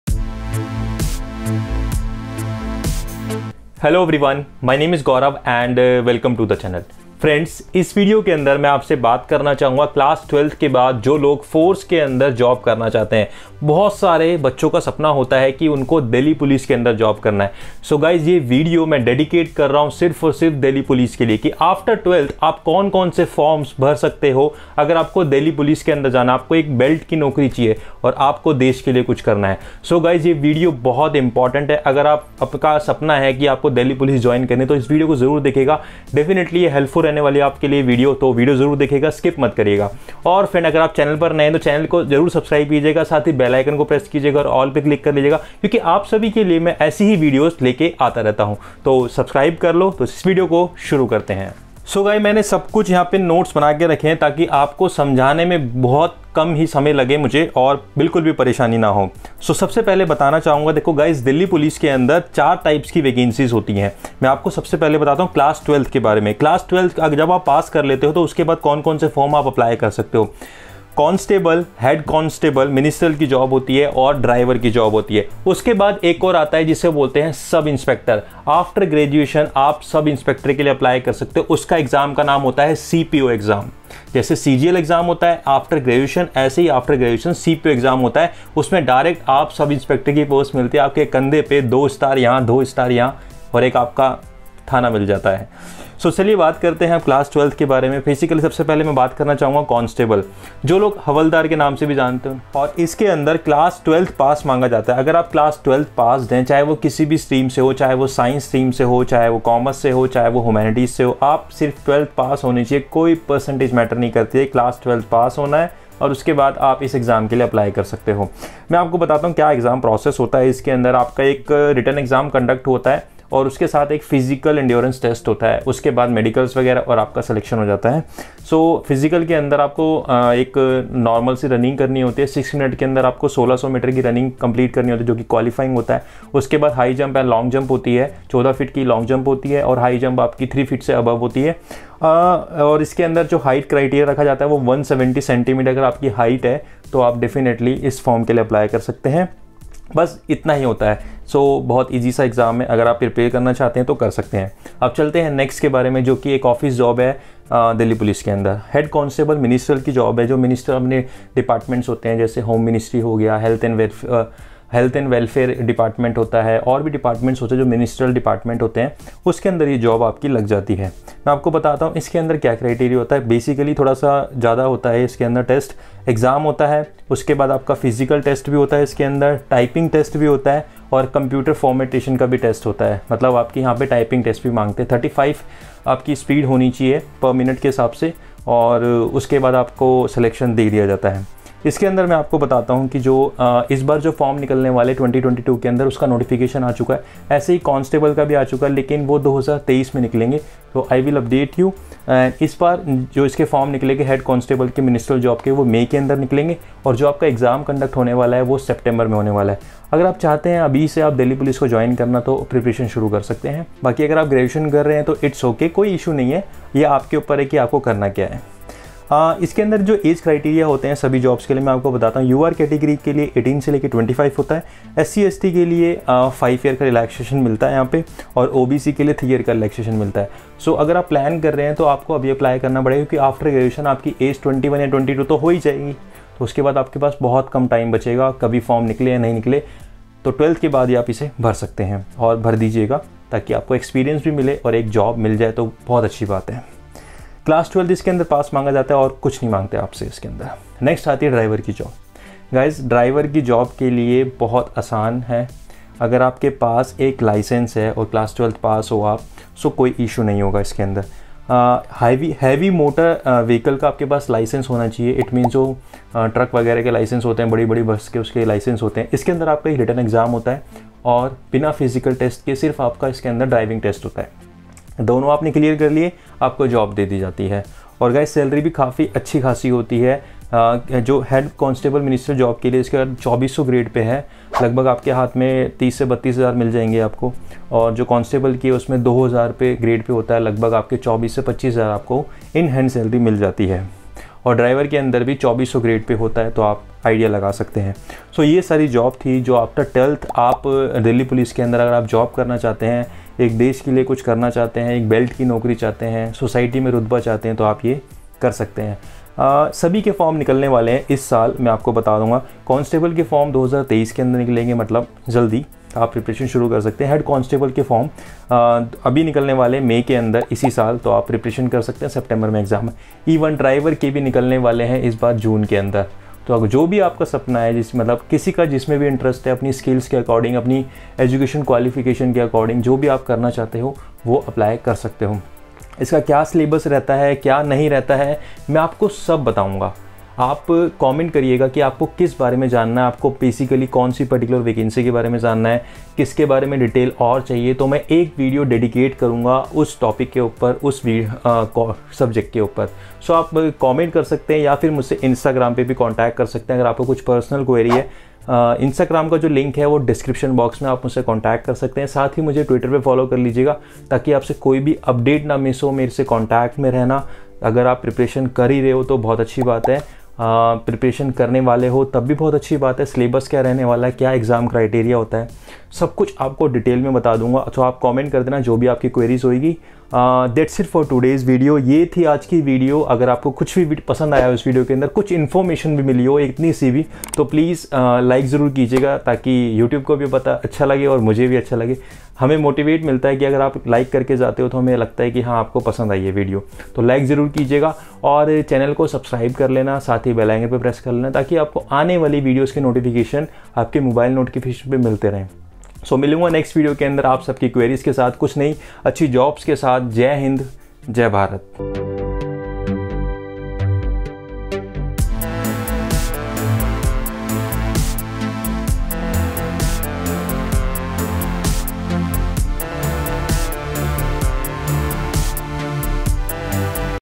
Delhi Police Jobs 2022 | After Class 12 | Biggest Opportunity
Today I m going to talk about Delhi Police after Class 12

I graduated form Delhi University(GGS) and currently work as PGT teacher with an Institute.

               "MY GEARS"

Camera : https
50 mm lens(Cinematic shot)
18-55mm Lens
Gorilla Pod
Audio mic
Shoutgun mic